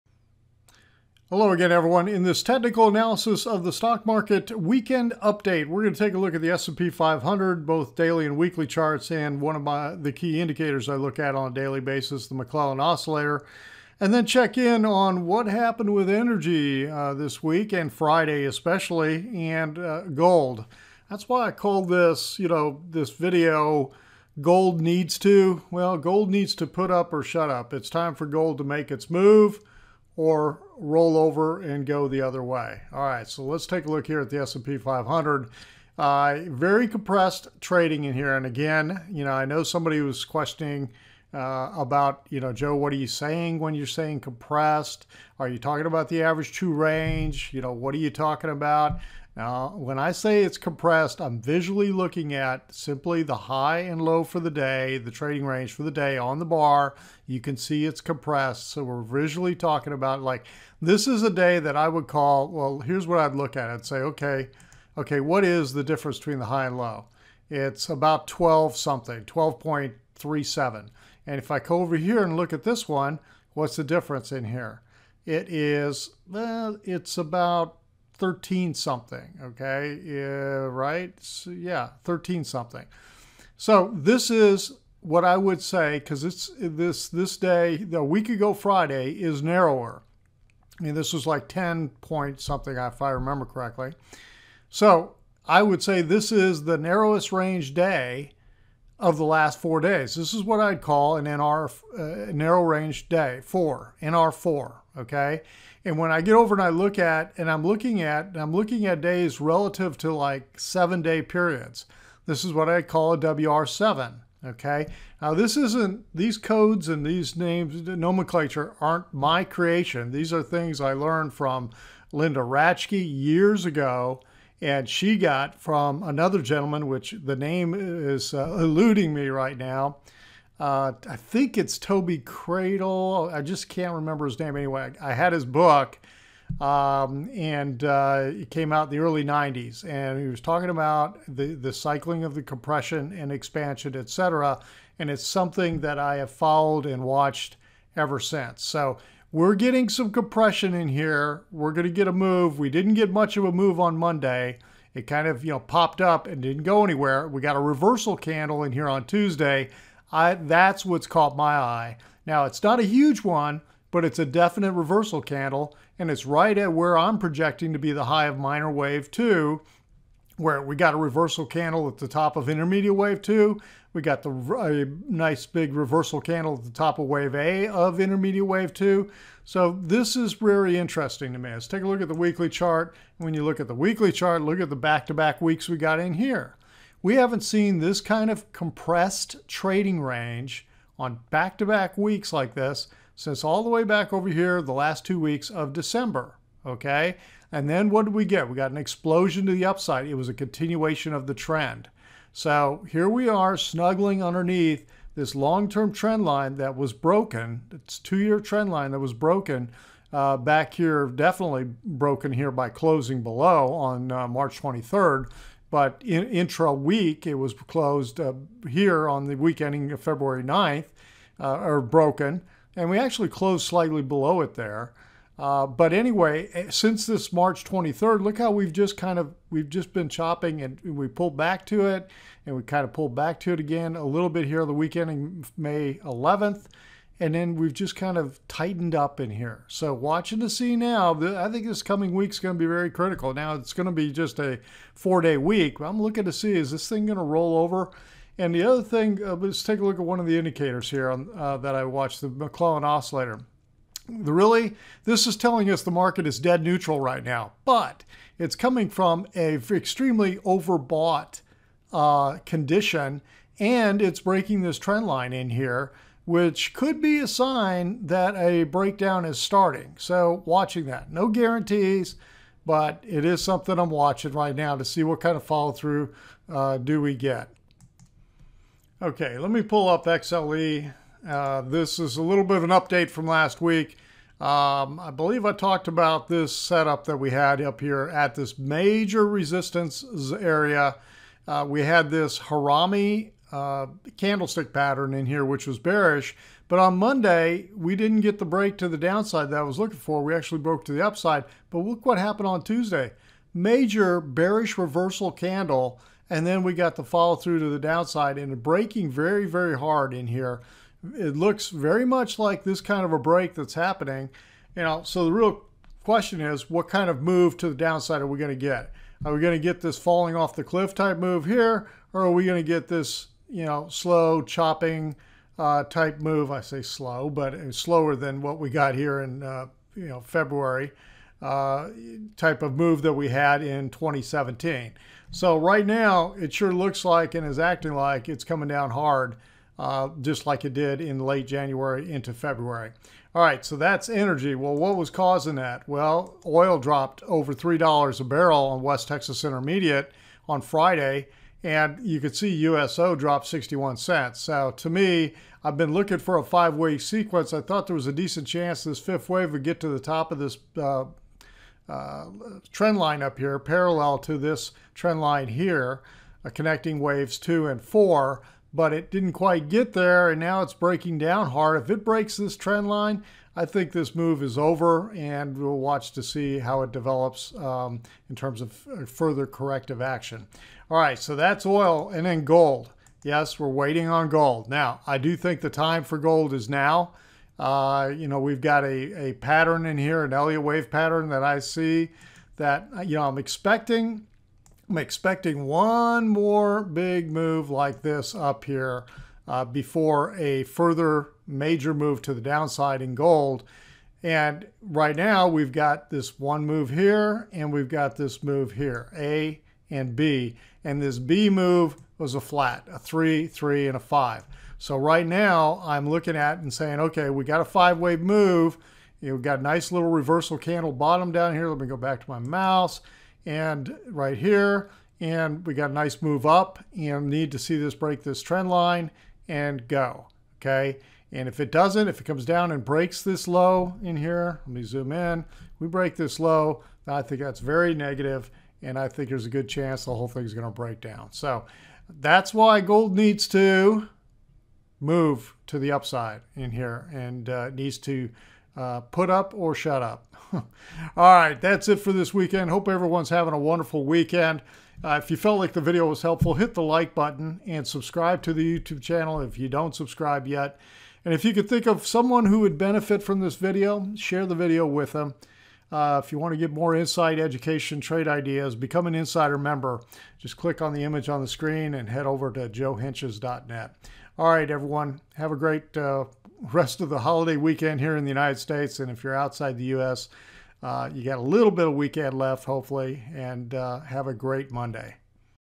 Technical Analysis of Stock Market | Gold Needs to...
In this technical analysis of stock market video I review the S&P 500 Index daily and weekly charts, one of my key indicators and then check back in on the Energy ETF (XLE), United States Oil (USO) and last but not least the Gold ETF (GLD).

How to Profit with Puts

Transform Your Trading Now!!